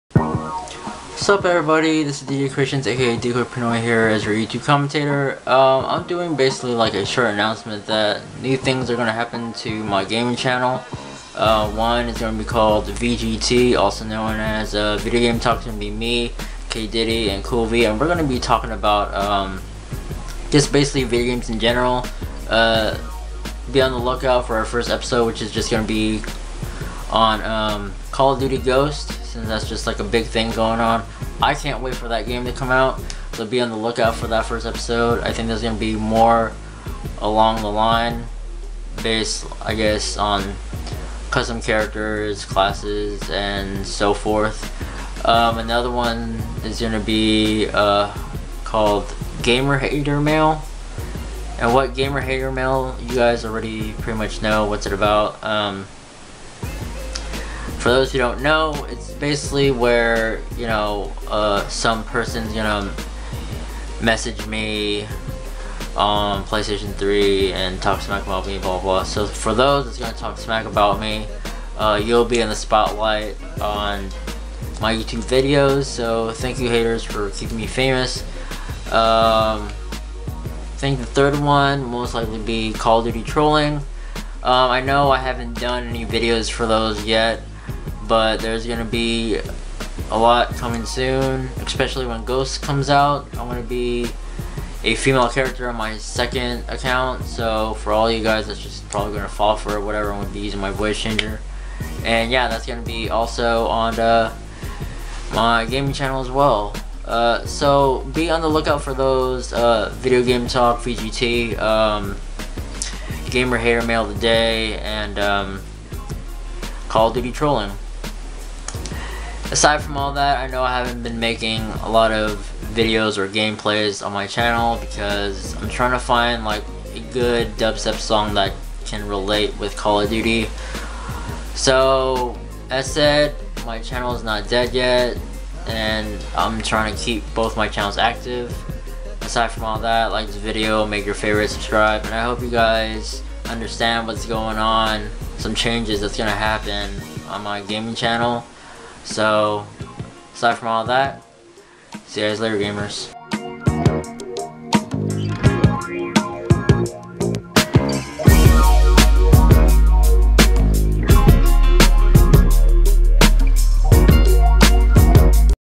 Few Changes On My Gaming Channel/Announcements
1) VGT - Video Game Talk: Pretty self explanatory though. Me and two of my friends will be talking about video games in general.

So watch what you say to me cause you will be on the spotlight :)

3) Trolling on call of duty: I know I have said this for a while, but I might be trolling when ghosts come out, so be patient and be on the lookout for it :)